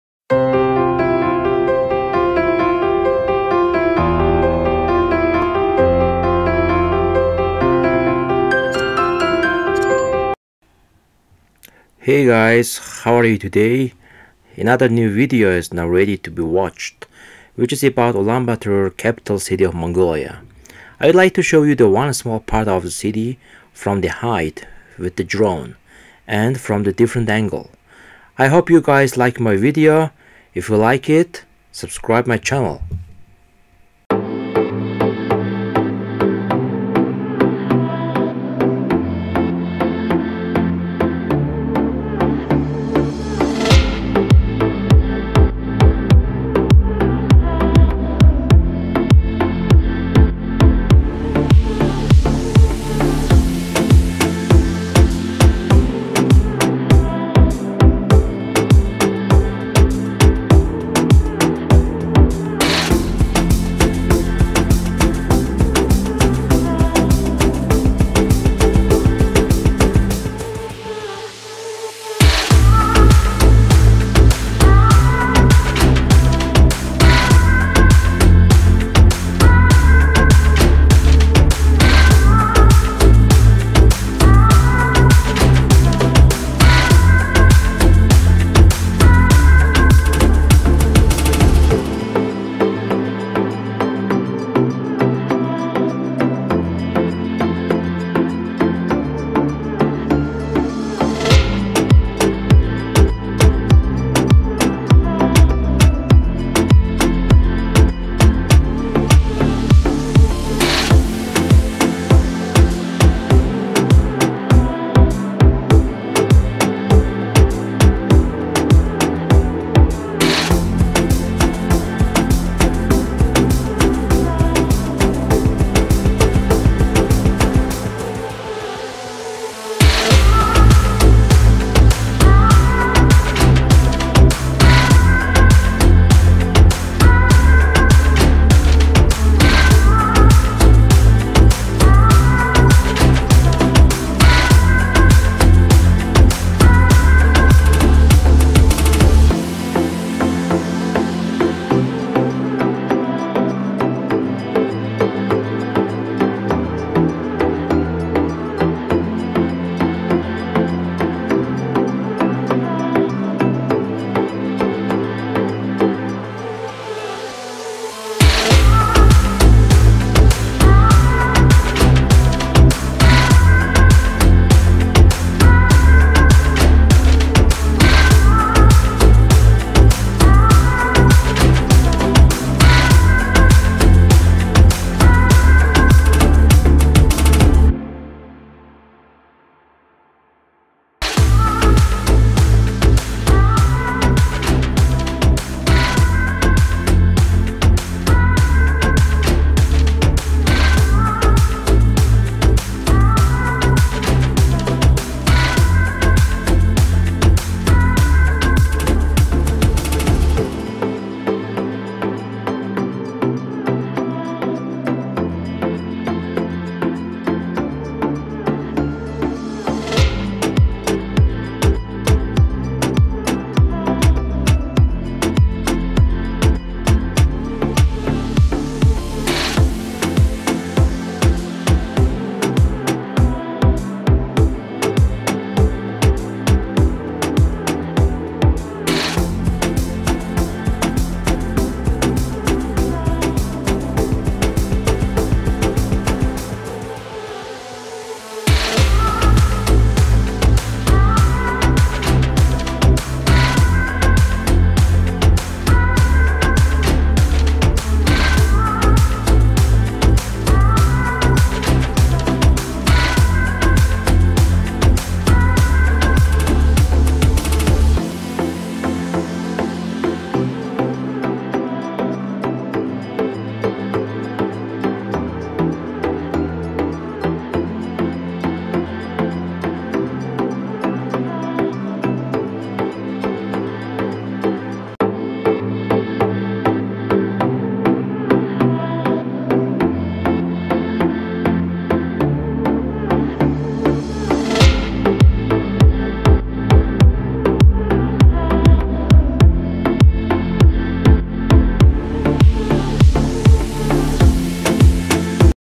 Ulaanbaatar city (part 1) - 1080 Full HD footage
This video is about one part of Ulaanbaatar, capital city of Mongolia. The footage was taken with drone. I hope you guys like my video.